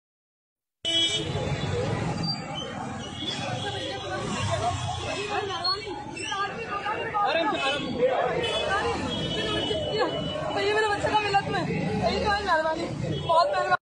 दिल्ली पुलिस को मेरा सलाम! बिना अपनी जान की परवाह किए, सबकी सहायता में लगें हैं। फिर भी एक समुदाय
दिल्ली पुलिस को मेरा सलाम!

बिना अपनी जान की परवाह किए, सबकी सहायता में लगें हैं। फिर भी एक समुदाय विशेष ने हेड कान्स्टबल रतन लाल जी की जान के ली।